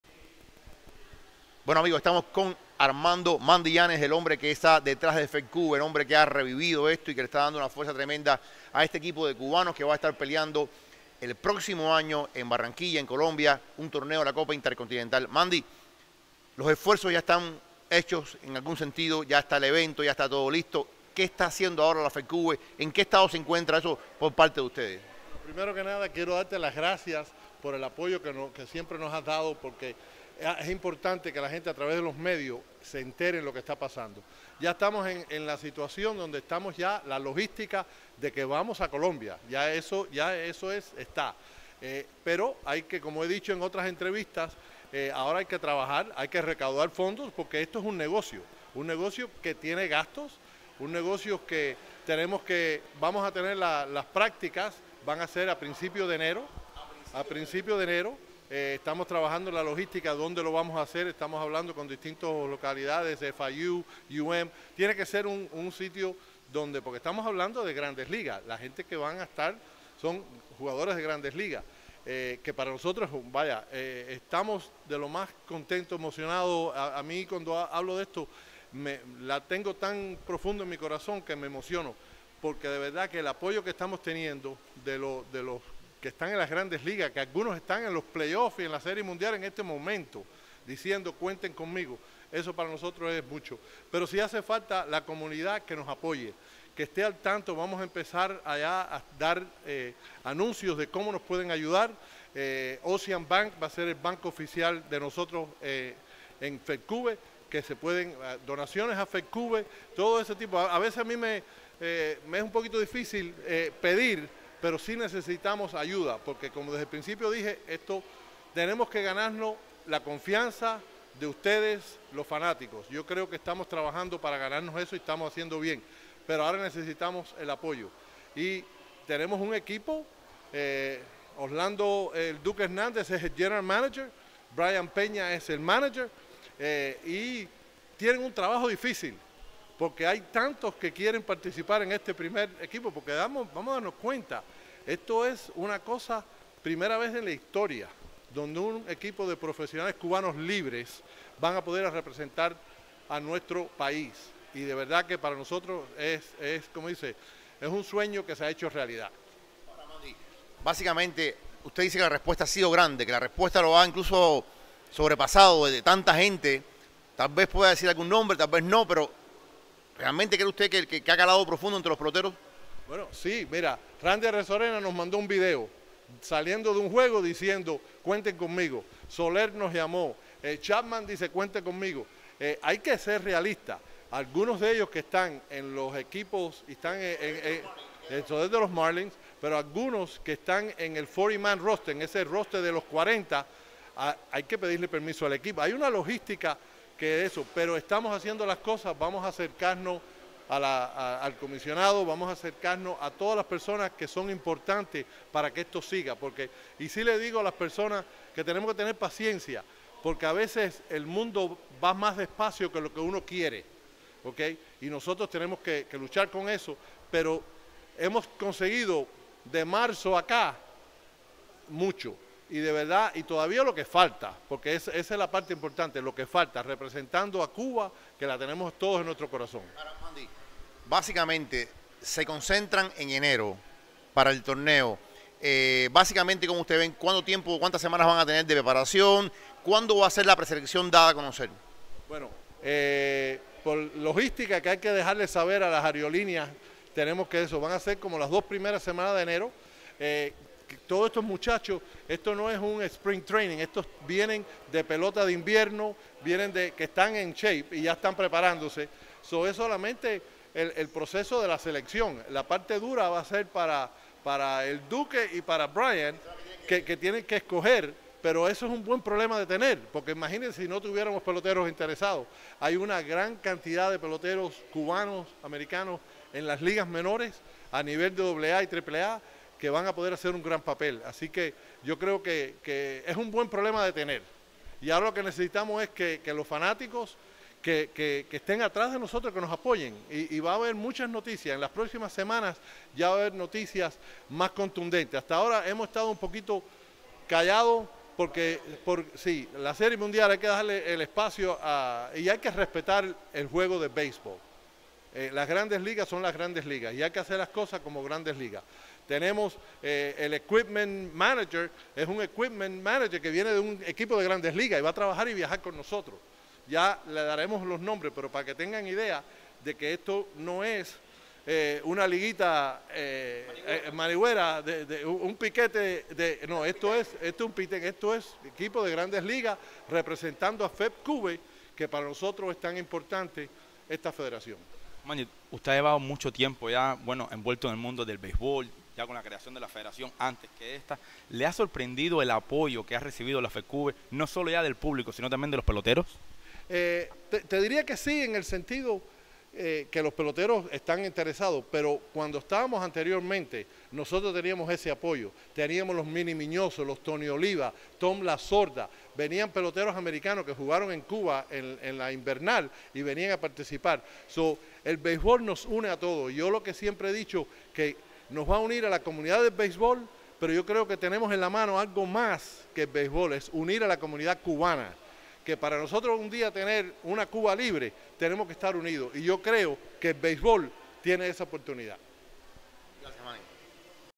El EQUIPO CUBA INDEPENDIENTE ultima detalles para su debut en enero
El Presidente de la Nueva Asociación de Peloteros Profesionales, Armando Llanes conversó con Pelota Cubana USA sobre los últimos detalles que ultima el EQUIPO CUBA INDEPENDIENTE para su debut en enero próximo.